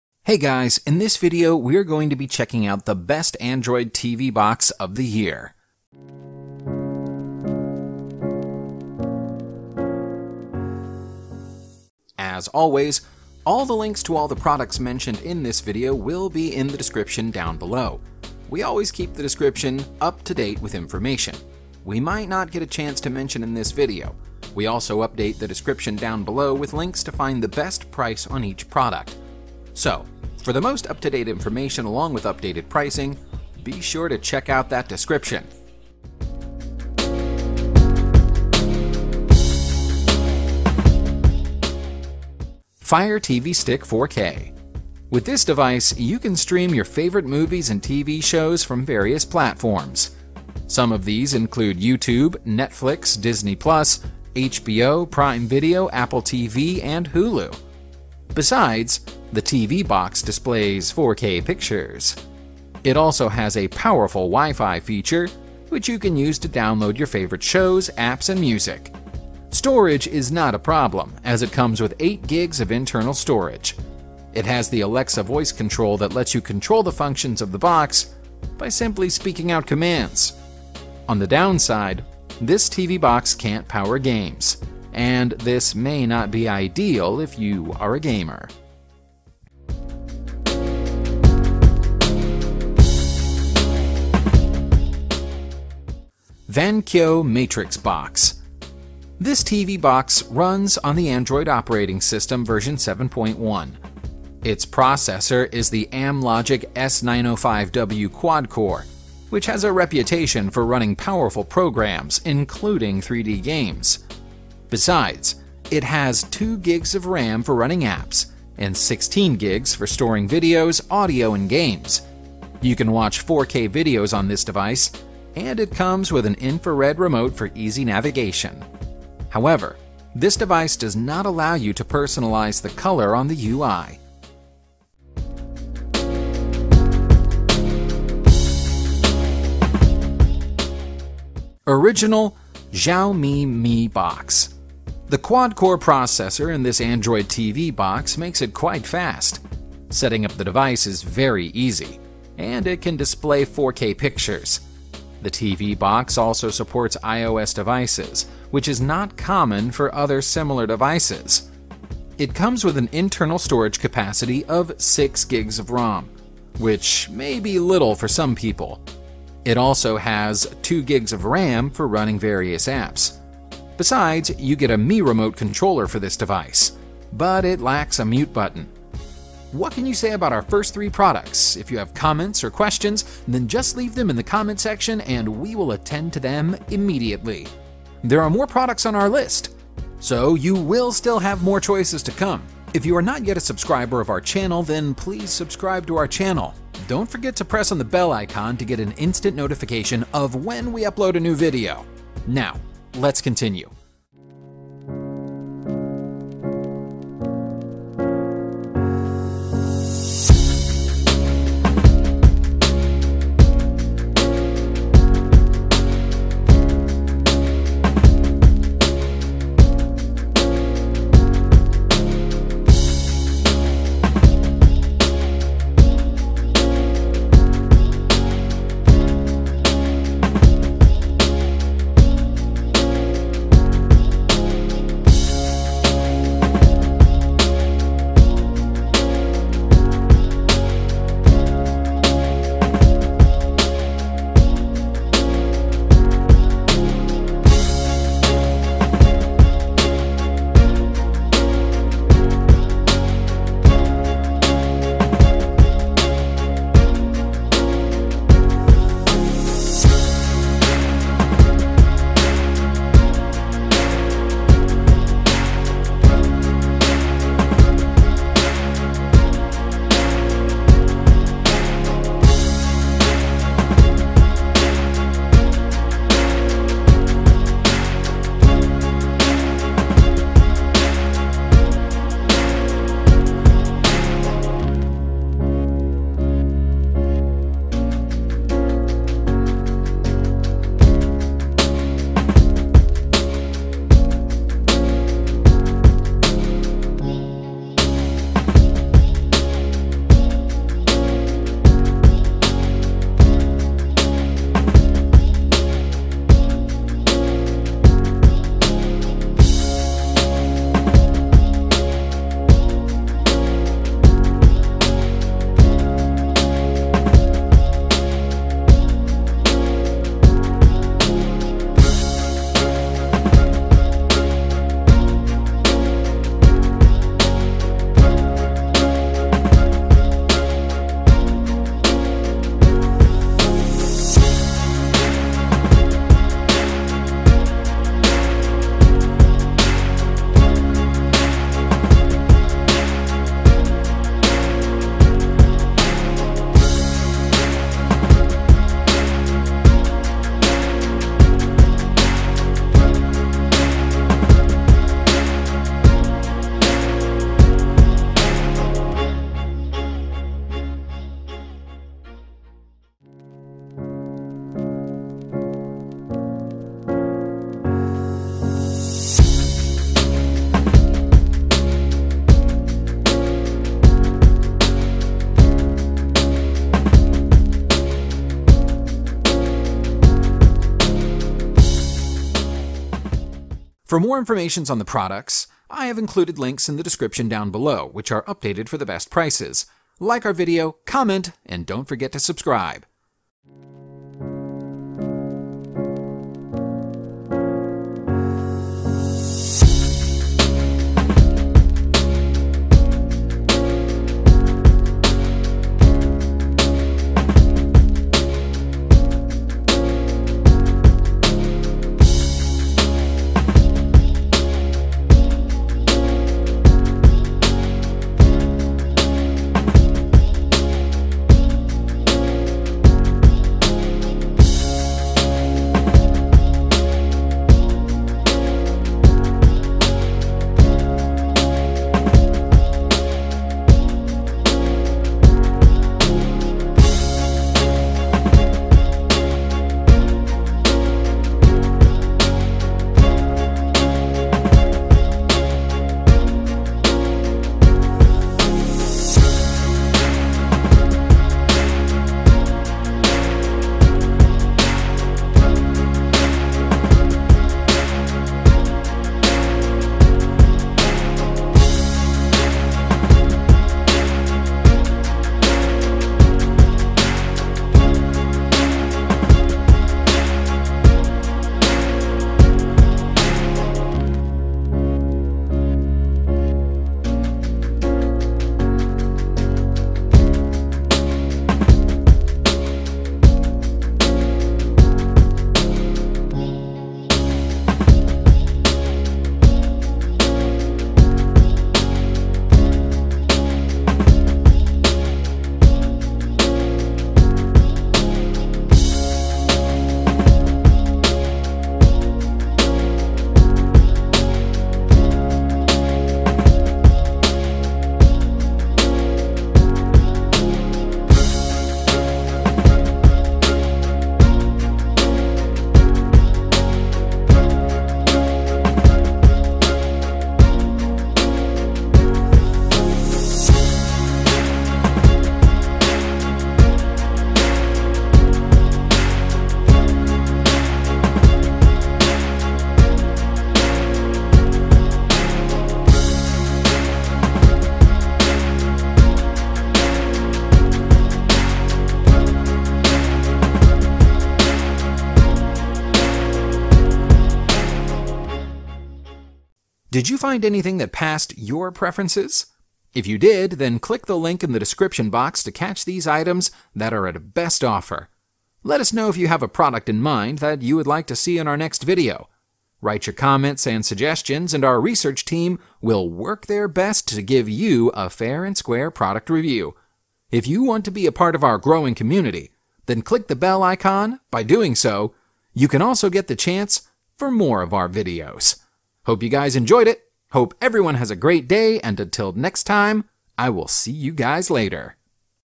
Best Android TV Box 2021 - 2023 - Supported 4K, WiFi ,Bluetooth & more!!
⇛Fire TV Stick 4K streaming device with Alexa built in, Dolby Vision, includes Alexa Voice Remote, latest release

⇛VANKYO MatrixBox X95A 4K Android TV Box, Ultra HD 2GB RAM 16GB ROM TV Streaming Player w/Amlogic S905W 64 Bits Quad Core Processor

⇛Original Xiaomi Mi Box - 4K Ultra HDR TV Streaming Media Player with Voice Search Remote (MDZ-16-AB) International Version

⇛Fire TV Stick with Alexa Voice Remote, streaming media player - Previous Generation
⇛MINIX NEO U1, 64-bit Quad-Core Media Hub for Android [2GB/16GB/4K/XBMC]. Sold Directly by MINIX Technology Limited.
⇛Android 9.0 TV Box,Pendoo TX6 Android TV Box 4GB DDR3 32GB EMMC Dual WiFi 2.4G+5G Bluetooth Quad Core 3D 4K Ultra HD H.265 USB3.0 Android TV Box
⇛NVIDIA SHIELD TV Pro Home Media Server
⇛EASYTONE T95Z Plus Android TV Box 3GB 32GB,Android 7.1 TV Box Amlogic Octa-Core,Dual-Band Wi-Fi 2.4/5.8G Smart Boxes Android Mini PC with Wireless Keyboard Remote (Backlit)
⇛EVANPO T95Z PLUS Android 7.1 TV BOX Amlogic S912 Octa-core CPU 3GB RAM 32GB ROM (Backlight Wireless Keyboard Included)
⇛Android 9.0 TV Box 4GB RAM 64GB ROM,MX10 PRO Android TV Box H6 Quad-core ARM Cortex-A53 CPU with 6K Image Decoding 2.4G WiFi 100M LAN USB3.0 H.265 VP9 Smart TV Box